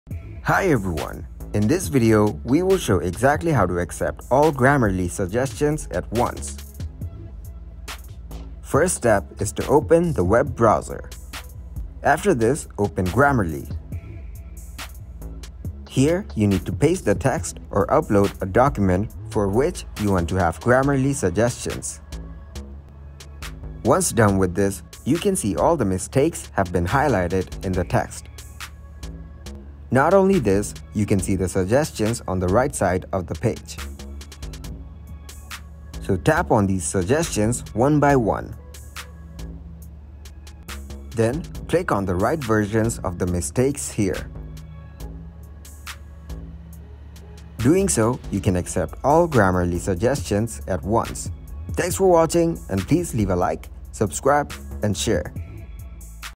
Is It Possible To Accept All Grammarly Suggestions At Once
In this video I will show you if Is It Possible To Accept All Grammarly Suggestions At Once

At the moment with free account I must say that this option is not possible at the moment. So you actually need to click one by one.